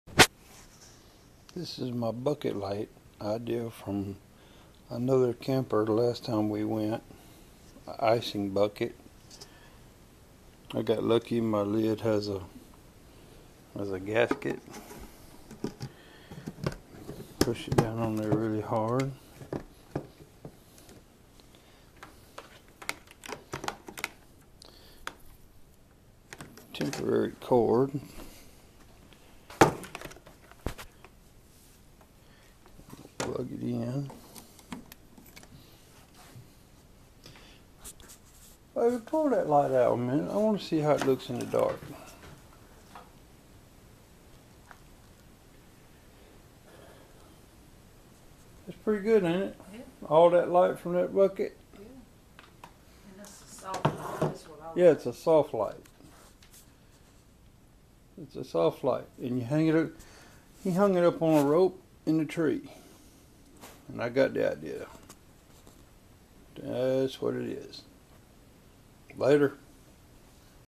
Bakery Icing bucket Light
I got the idea from another camper who got it from another camper   :)
you hang it from a rope in a tree to provide light for your campsite.  It stays dry.  It's a soft light.  and it works great.  I used an energy efficient 20w which states it's equal to a 75W.so it's pretty bright.